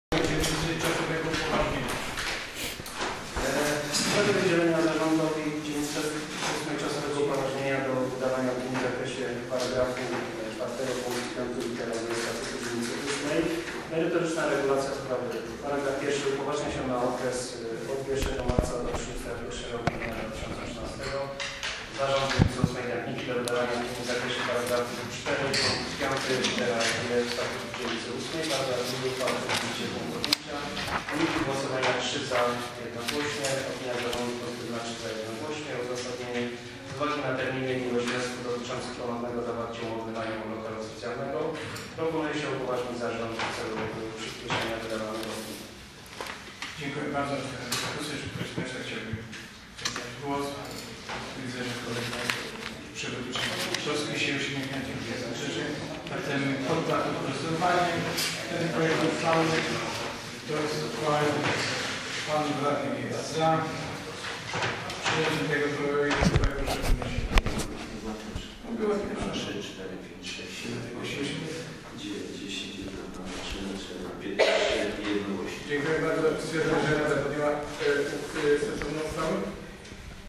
RD_VIII_27-02-2013_6 Uchwała ws. udzielenia Zarządowi czasowego upoważnienia do wydawania opinii
Obrady XXXVII sesji Rady Dzielnicy VIII Dębniki z dn. 27 lutego 2013 r. Uchwała ws. udzielenia Zarządowi Dzielnicy VIII czasowego upoważnienia do wydawania opinii w zakresie par. 4 pkt. 5 lit. g. Statutu Dzielnicy VIII.